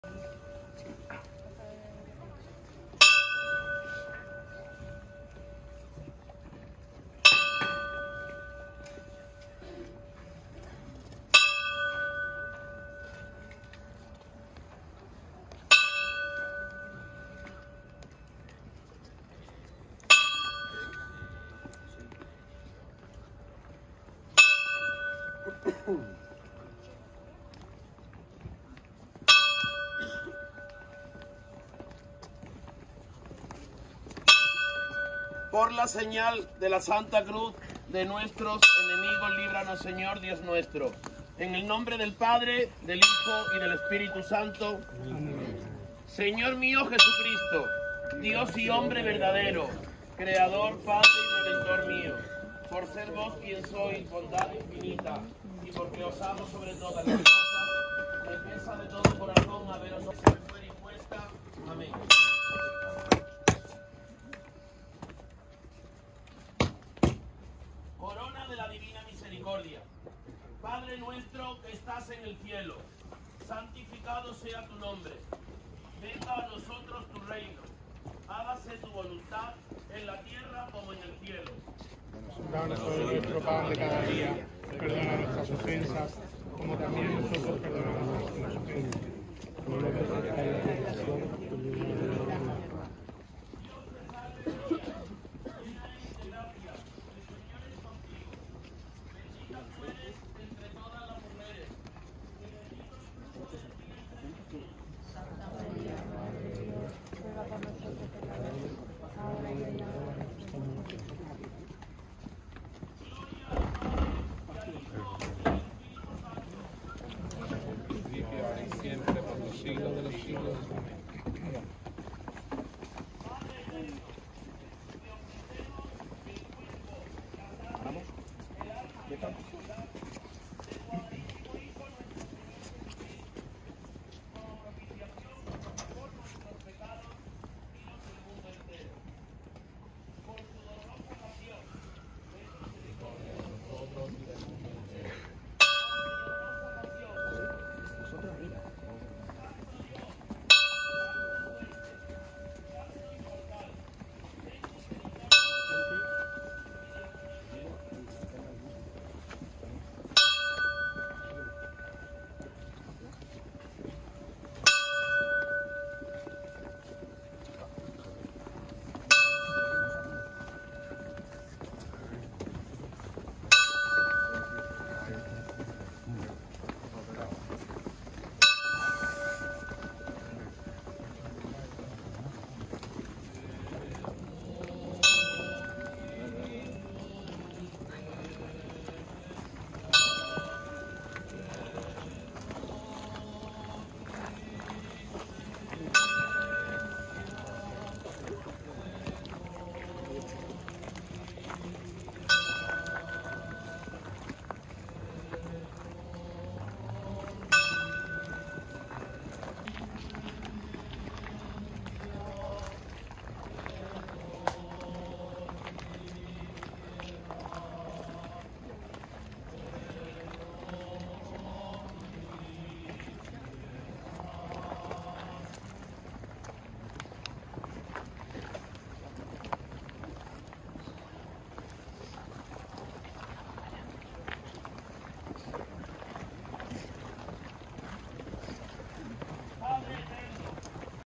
Mortaja | Traslado a la Victoria | Nazareno Cofrade
Vídeo del traslado del Cristo Yacente de la Paz y Unidad del Monte Calvario a la Victoria para su quinario en la primera semana de cuaresma